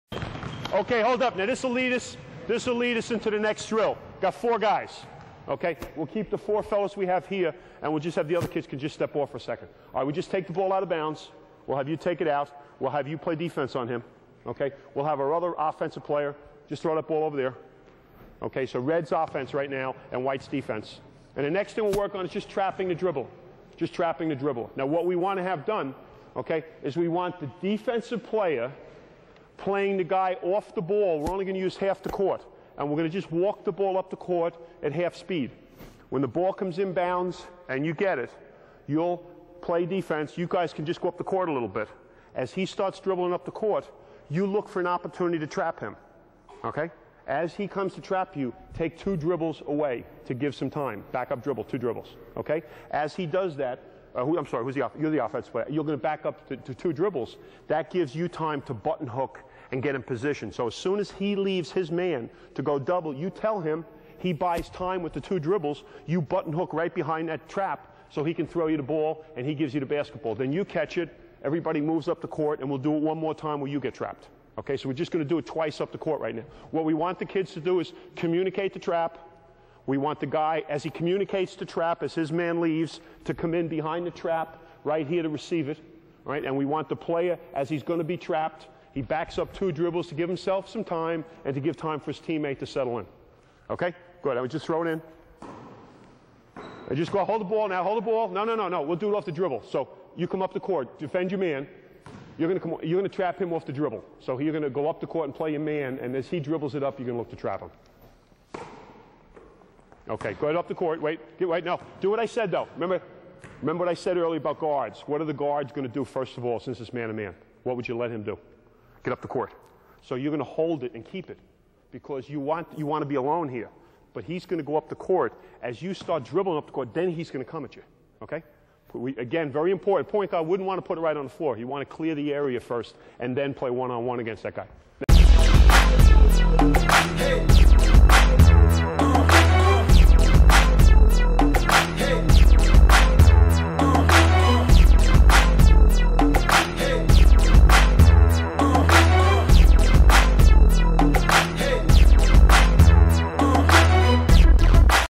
Point Guard Play with Bob Hurley Preview
The key to making any team great revolves around the ability of the point guard to get the job done. Coach Bob Hurley, Sr. takes you through various drills and an off-season workout that will help any player become a better ballhandler, but Hurley also talks about the key ingredients it takes to become a great point guard. He tells how the point guard can demoralize the opponents and will help you understand the mentality of playing the position. This video was done in a clinic setting with Coach Hurley talking to a group of coaches and directing on-court demonstrations. Running Time: 70 Minutes.